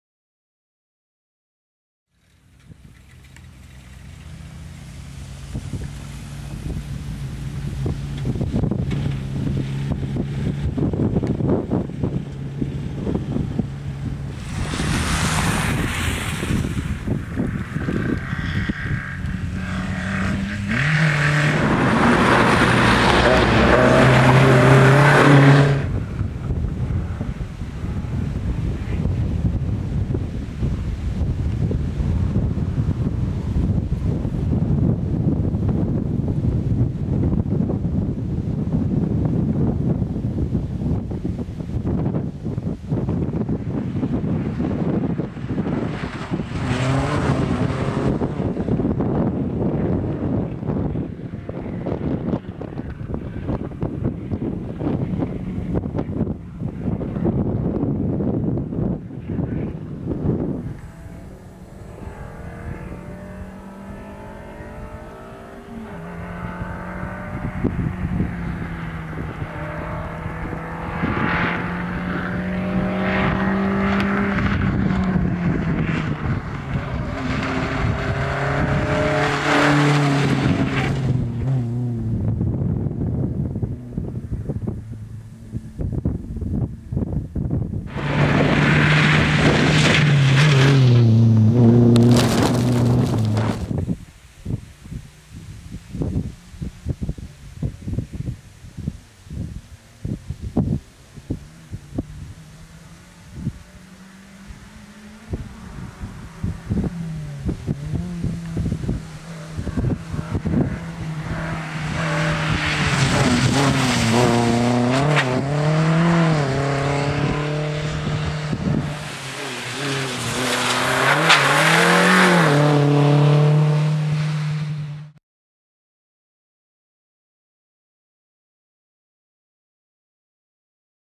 Lisowski/Frąckowiak (MG ZR 160) - IV Runda MOTUL Rallyland Cup (01.09.2013)
Przejazdy załogi Radosław Lisowski/Piotr Frąckowiak podczas czwartej rundy MOTUL Rallyland Cup (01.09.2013).